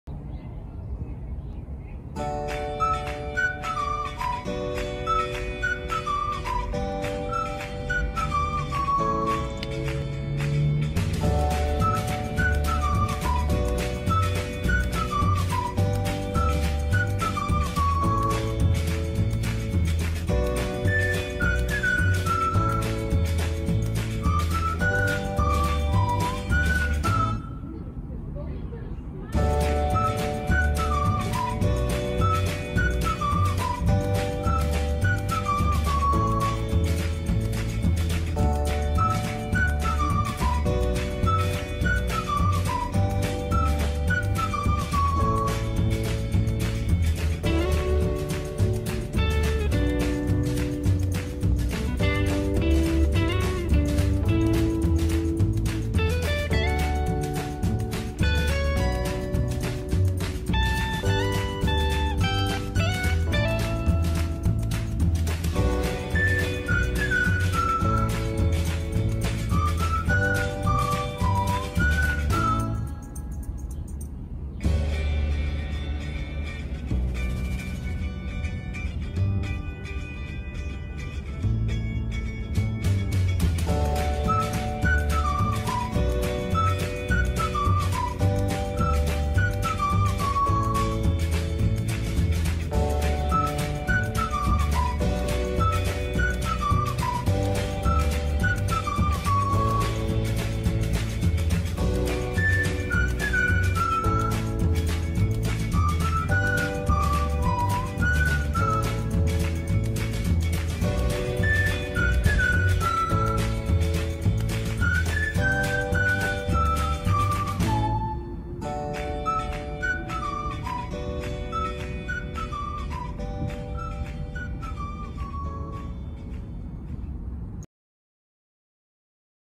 HOME GARDENING 🌱 MY GREEN BEANS ARE BLOOMING LifeInUSA FilipinoAmerican 🇵🇭🇺🇸
My green beans plant is blooming 😍 It has lots of flowers.